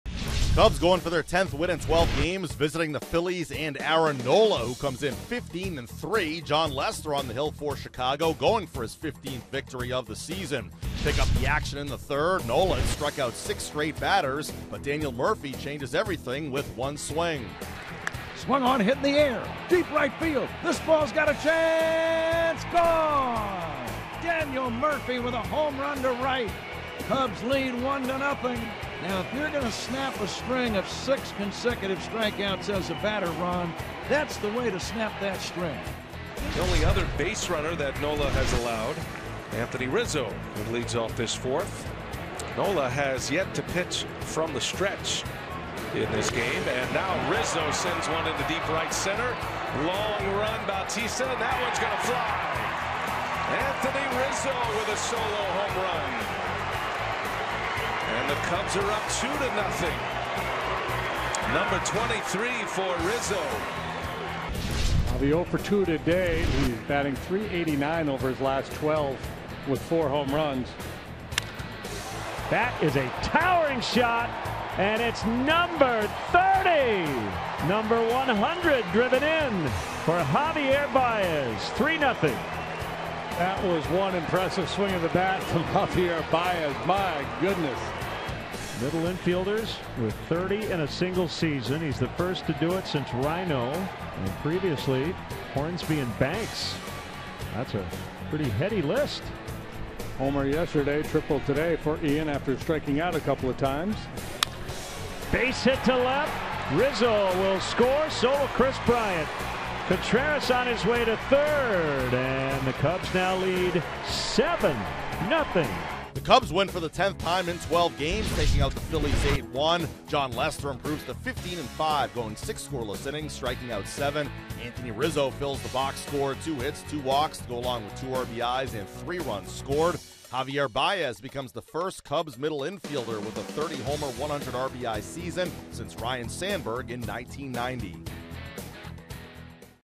Baez, Lester lead Cubs to series win: 9/2/18
Daily Recap: Javier Baez reached the 30-homer and 100-RBI mark, while Jon Lester went six scoreless for his 15th win of the season